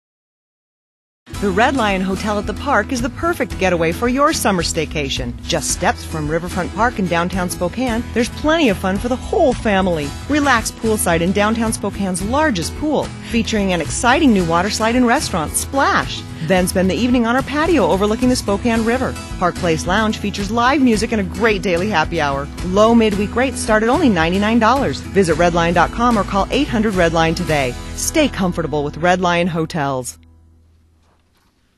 Red Lion Hotel at the Park is offering summer Staycations
Check out redlion.com for great rates on mid-week summer Staycations. Get away without going away and enjoy our patio, poolside dining and brand new water slide.